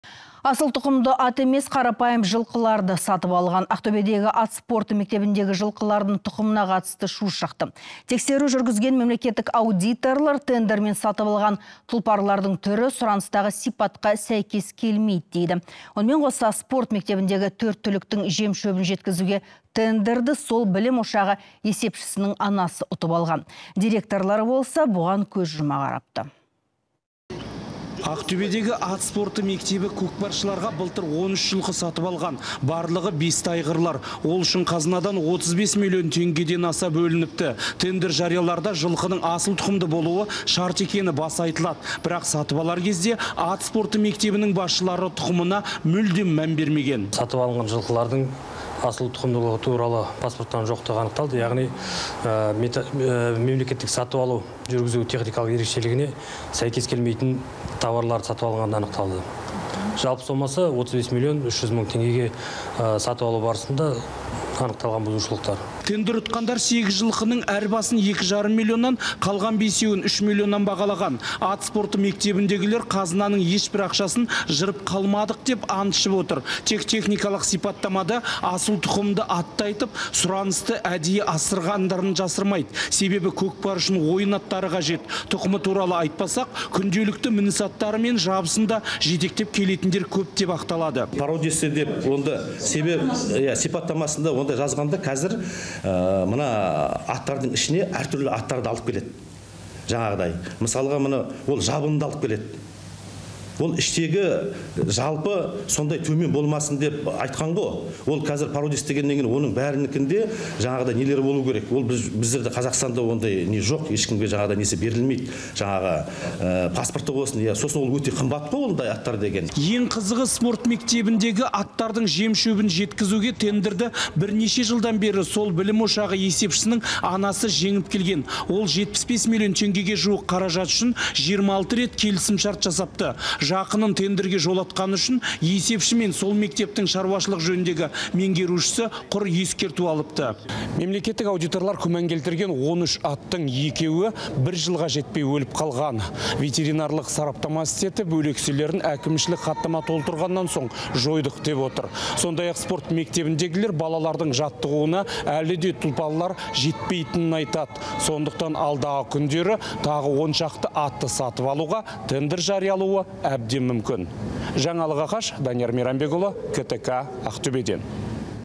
«Нелері болуы керек, ондай не жоқ, ешкімге несі берілмейді»
Асыл тұқымды ат емес, қарапайым жылқыларды сатып алған. Ақтөбедегі ат спорты мектебіндегі жылқылардың тұқымына қатысты шу шықты.